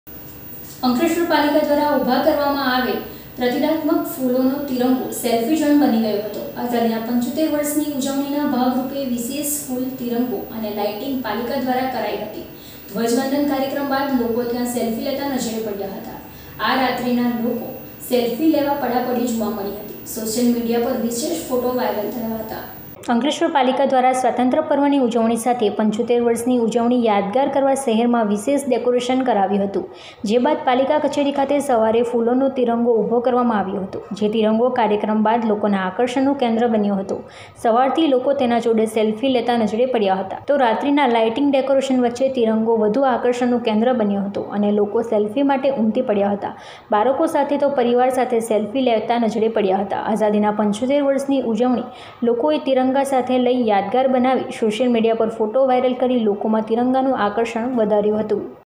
માઁ ટીવી 16.08.2022 પાલિકા દ્વારા ઊભા કરવામાં આવેલ પ્રતીકાત્મક ફૂલો નો તિરંગો સેલ્ફી ઝોન બન્યો
આકર્ષણ નું કેન્દ્ર : અંકલેશ્વર પાલિકા દ્વારા ઊભા કરવામાં આવેલ પ્રતીકાત્મક ફૂલો નો તિરંગો સેલ્ફી ઝોન બન્યો

આઝાદી ના 75 વર્ષ ની ઉજવણી ના ભાગ રૂપે વિશેષ ફૂલો તિરંગો અને લાઈટીંગ પાલિકા દ્વારા કરાઈ હતી. : ધ્વજવંદન કાર્યક્રમ બાદ લોકો ત્યાં સેલ્ફી લેતા નજરે પડ્યા : રાત્રી ના લોકો સેલ્ફી લેવા પડાપડી જોવા મળી : સોશિયલ મીડિયા પર વિશેષ ફોટો વાયરલ થયા